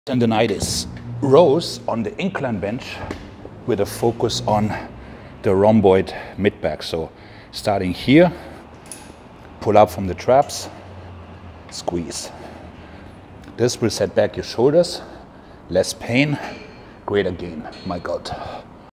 Fix Shoulder Tendonitis! | How To Prevent Your Shoulder Tendonitis | Shoulder Tendonitis Exercises
Tendonitis rose on the incline bench with a focus on the rhomboid mid-back. So starting here pull up from the traps squeeze this will set back your shoulders less pain great again, my god.

Prevention beats rehab!